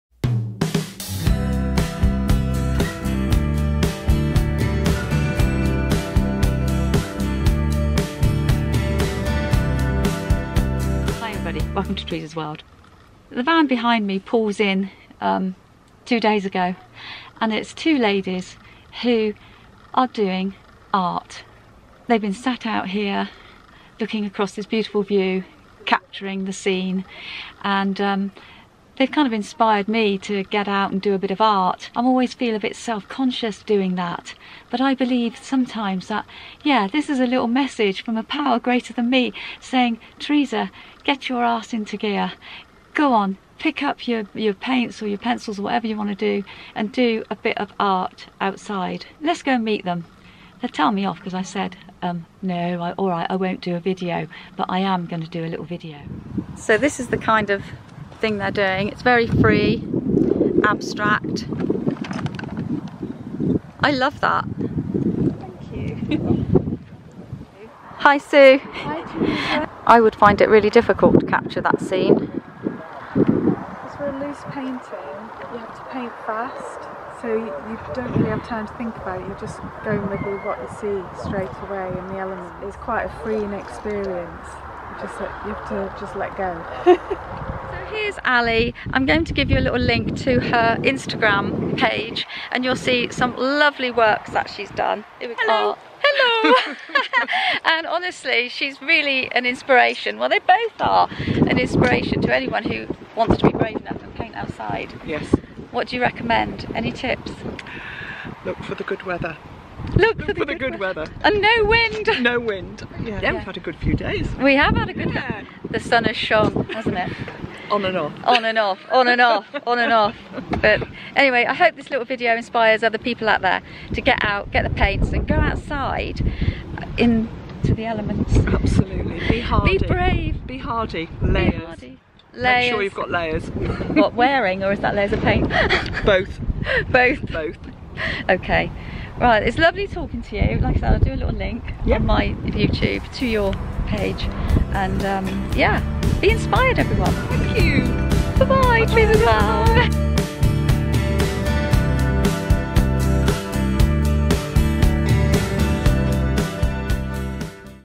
Art be inspired to paint outside - outdoor art - acrylics
Whilst in the Peak District, I met 2 inspiring artists who were braving the elements and painting outdoors. I was inspired! I hope this video inspires you too.